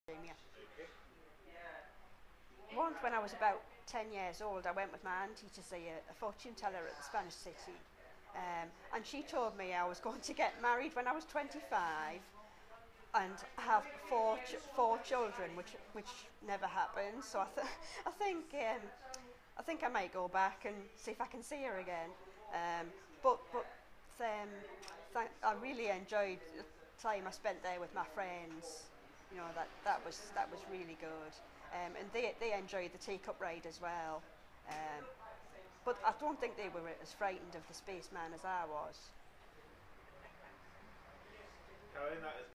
Caroline's memories part 2
Caroline talks about the time she saw a fortune teller at the Spanish City.